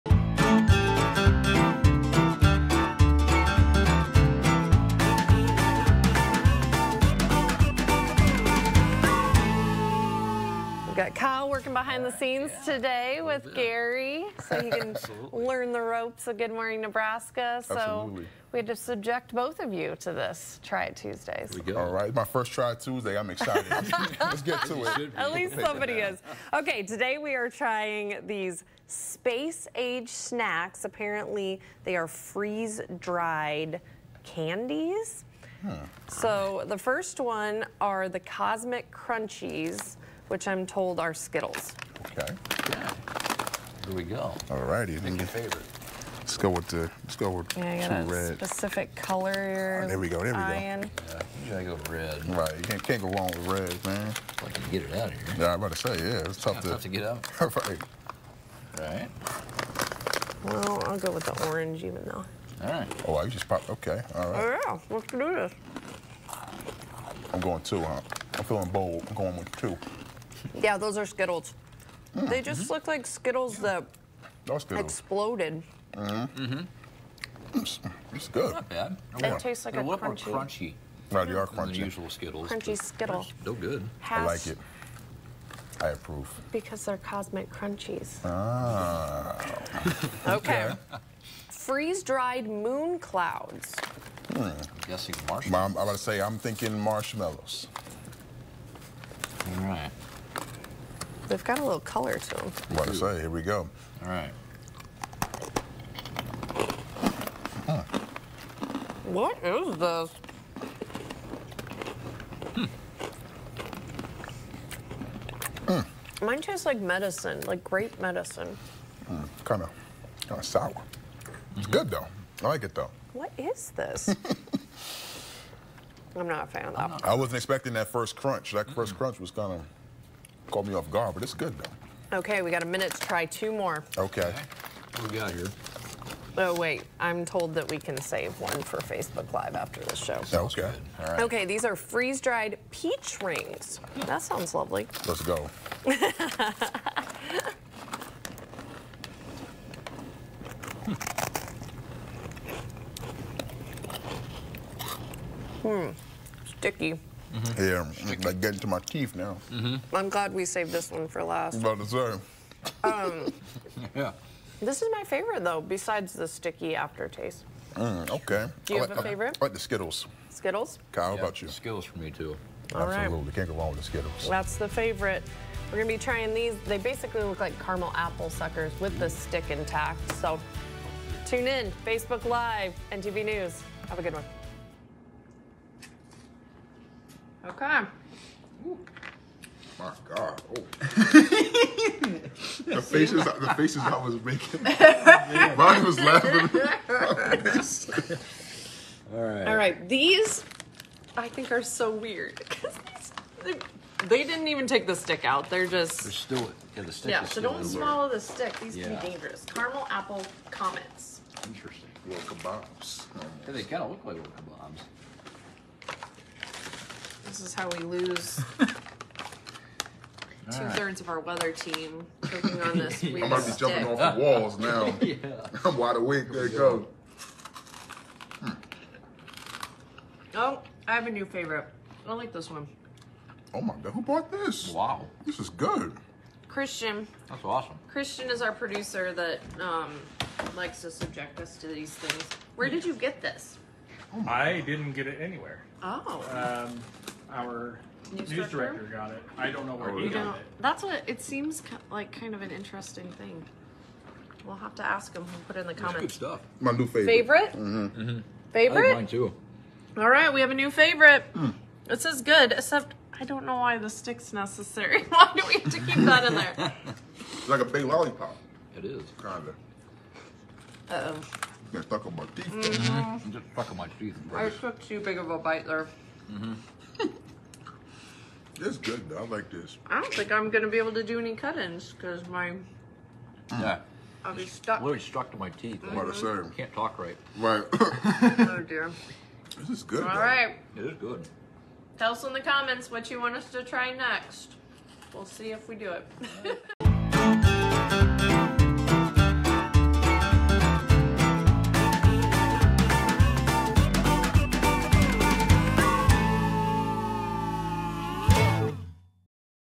Try It Tuesdays - Freeze Dried Candy!
This week, Sara, Kyle, and Gary try candy, but not just any kind!

Follow Good Morning Nebraska!

Follow Sara Kirkley!

Follow Meteorologist Kyle Kleckner!

Follow Gary Robinson, Jr.